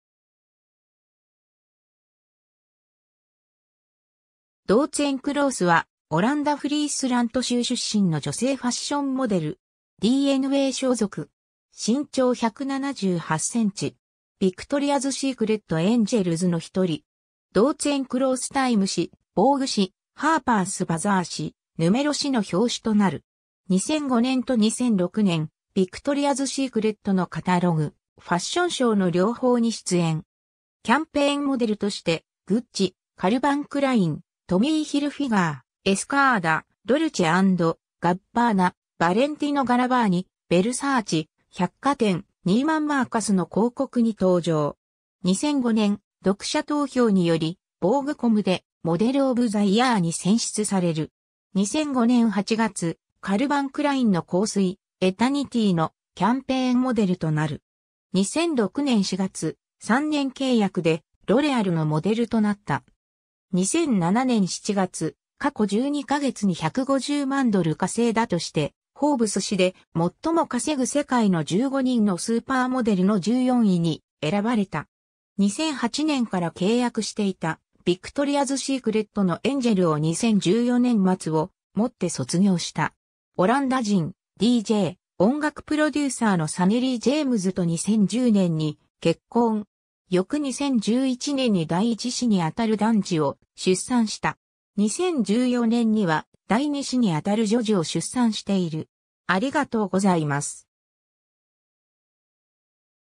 ドウツェン・クロース
ヴィクトリアズ・シークレットのモデル
ドウツェン・クロース（Doutzen Kroes、オランダ語：ダウツン・クルーシュ、1985年1月23日 - ）は、オランダ・フリースラント州出身の女性ファッションモデル。
 DNA所属。
身長178センチ。
ヴィクトリアズ・シークレット・エンジェルズの一人。
 ドウツェン・クロース（2009年） タイム誌、ヴォーグ誌、ハーパース・バザー誌、ヌメロ誌の表紙となる。
2005年と2006年、ヴィクトリアズ・シークレットのカタログ、ファッションショーの両方に出演。
キャンペーンモデルとして、グッチ、カルバン・クライン、トミーヒルフィガー、エスカーダ、ドルチェ&ガッバーナ、ヴァレンティノ・ガラヴァーニ、ヴェルサーチ、百貨店ニーマン・マーカスの広告に登場。
 2005年、読者投票によりヴォーグ・コムでモデル・オブ・ザ・イヤーに選出される。
2005年8月、カルバン・クラインの香水『エタニティー』のキャンペーンモデルとなる。
2006年4月、3年契約でロレアルのモデルとなった。
 2007年7月、過去12ヶ月に150万ドル稼いだとして、フォーブス誌で『最も稼ぐ世界の15人のスーパーモデル』の14位に選ばれた。
 2008年から契約していたヴィクトリアズ・シークレットのエンジェルを2014年末をもって卒業した。
 オランダ人DJ・音楽プロデューサーのサネリー・ジェームズと2010年に結婚。
翌2011年に第1子にあたる男児を出産した。
2014年には第2子にあたる女児を出産している。